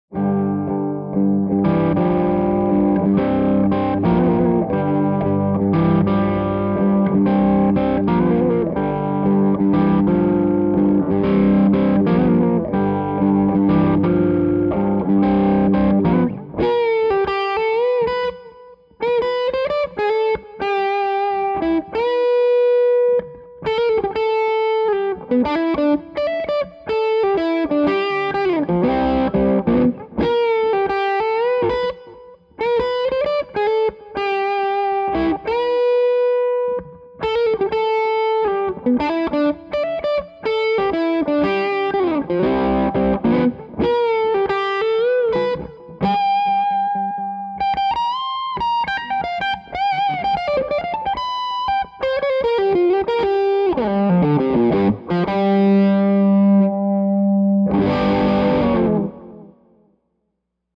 Vertex Ultra-phonix HRM Quick Playthrough
Whats up guys, got to take the new Vertex Ultraphonix HRM for a spin yesterday. Super Robben Ford Vibes!